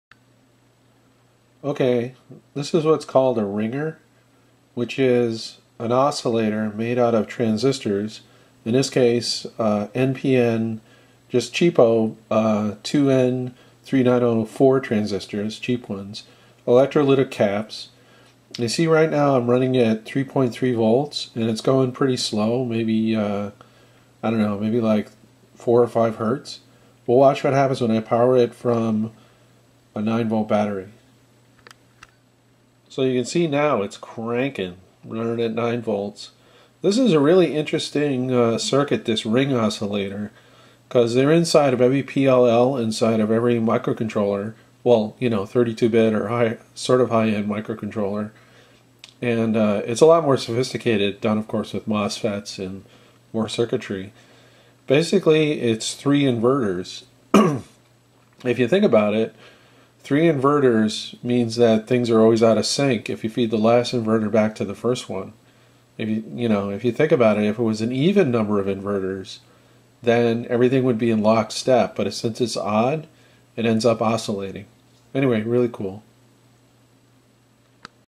Ring Oscillator
Breadboard demonstration of a ring oscillator.